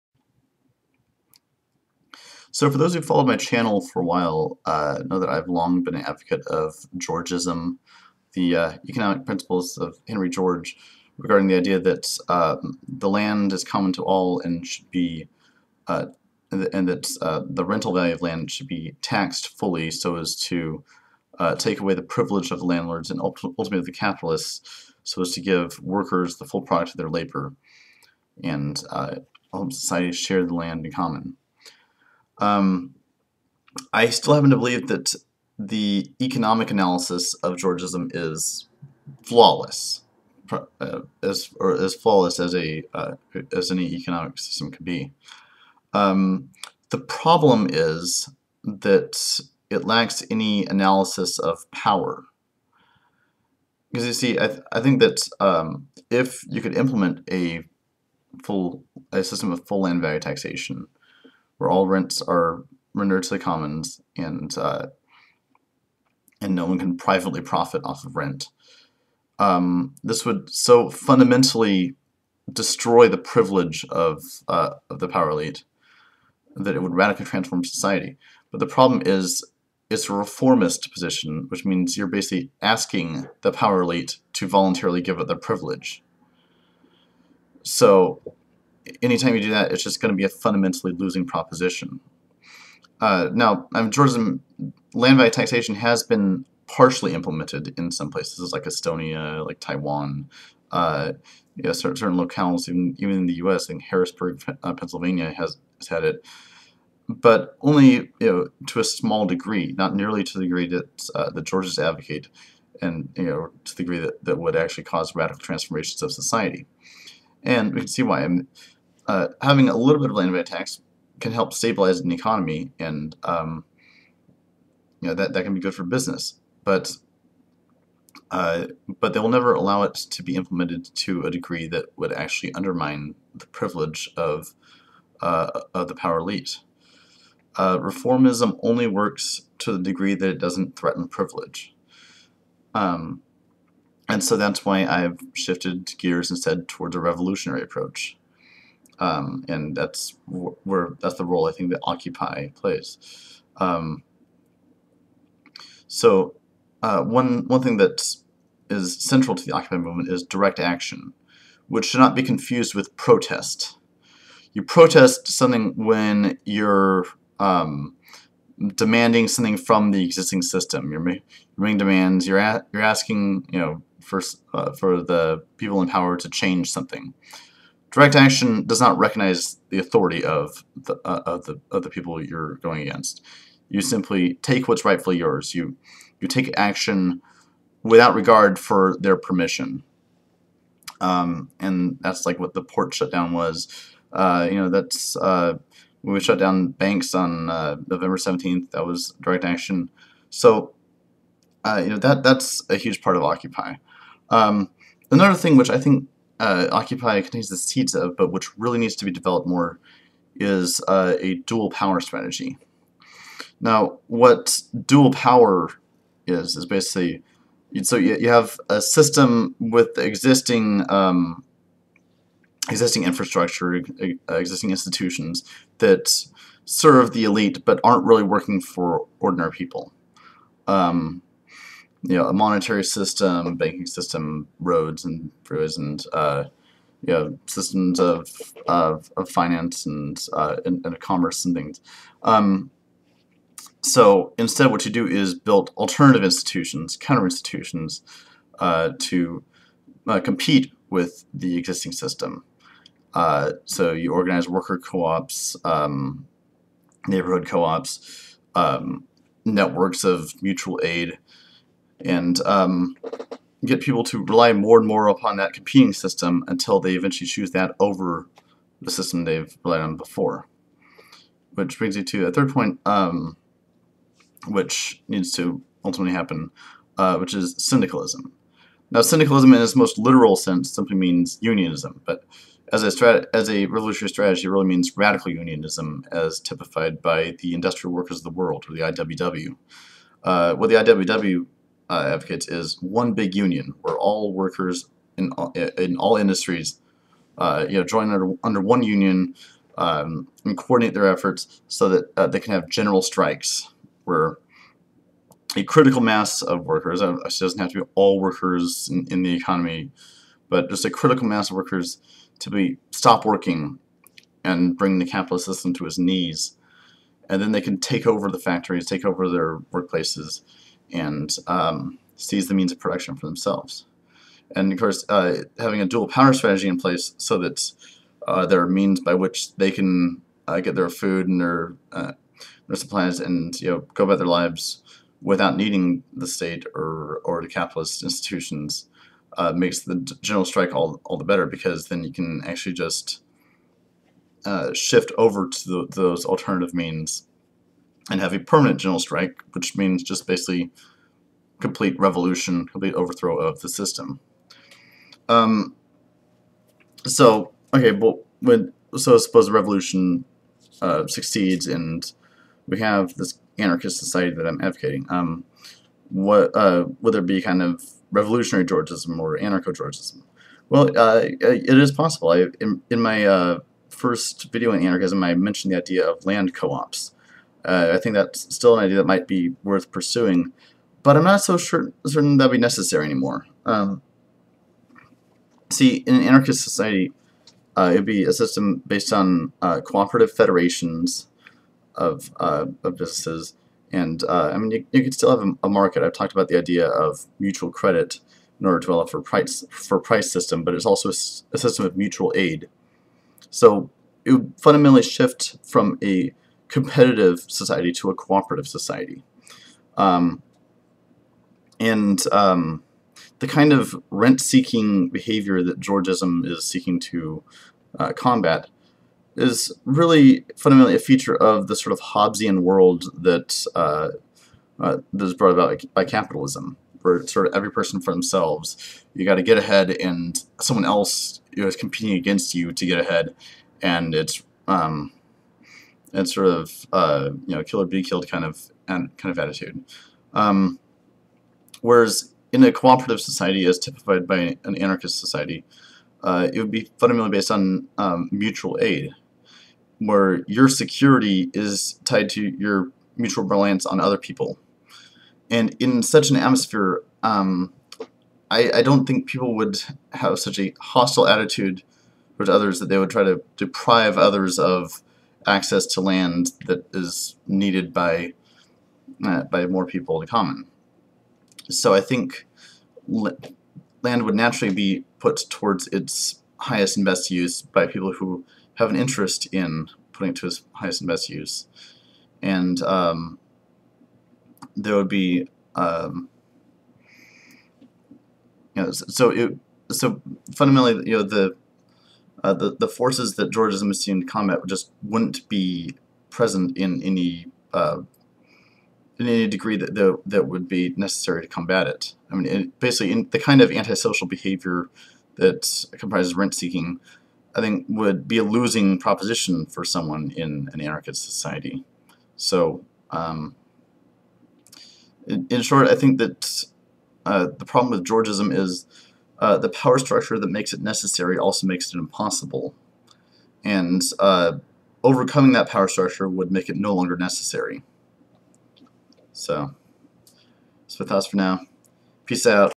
Power, Class, and Privilege
A sociological critique of Georgism based on conflict theory, plus a brief explanation of competitive versus cooperative societies.